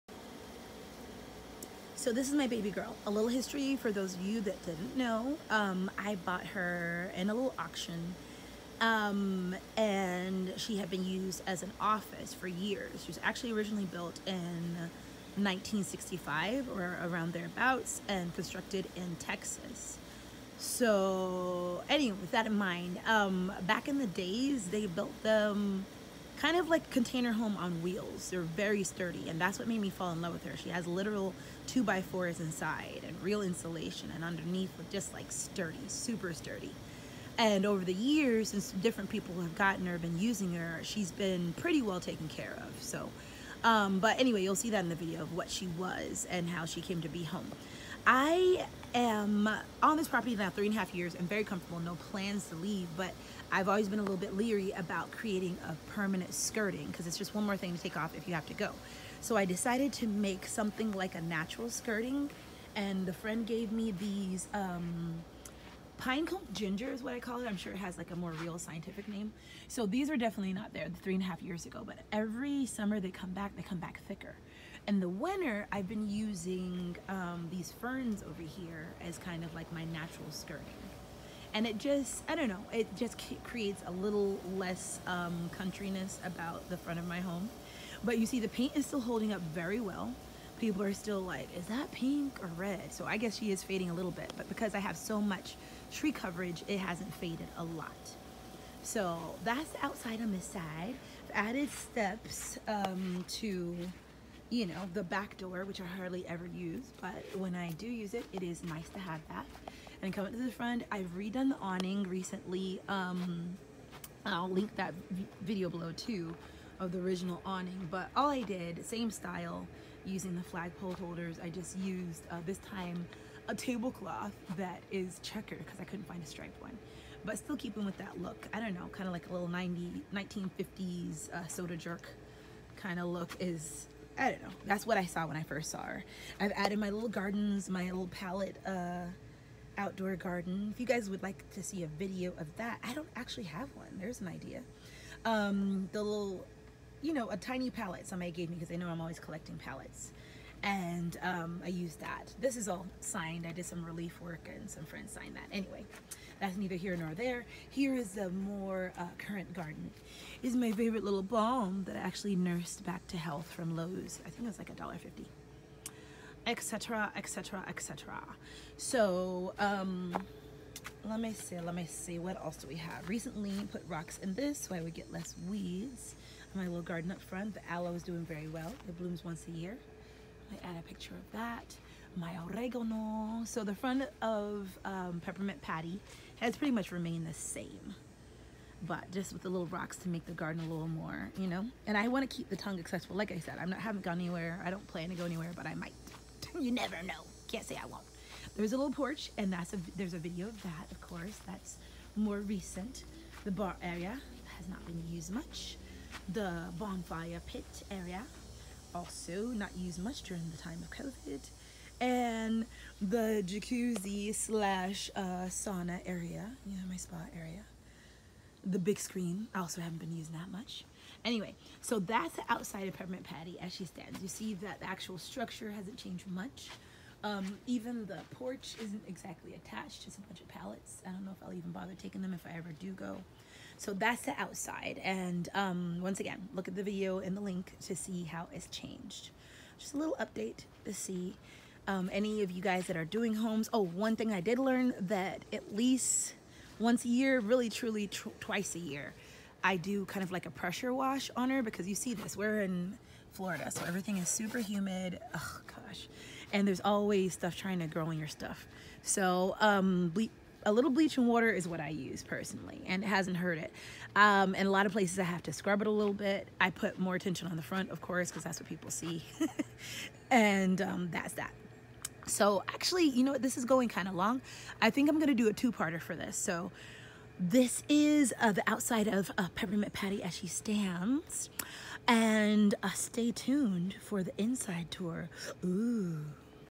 Tiny house update 3 years later
I have been living in my tiny house/camper/container home on wheels for 3.5 years now. What has changed, and what have I learned???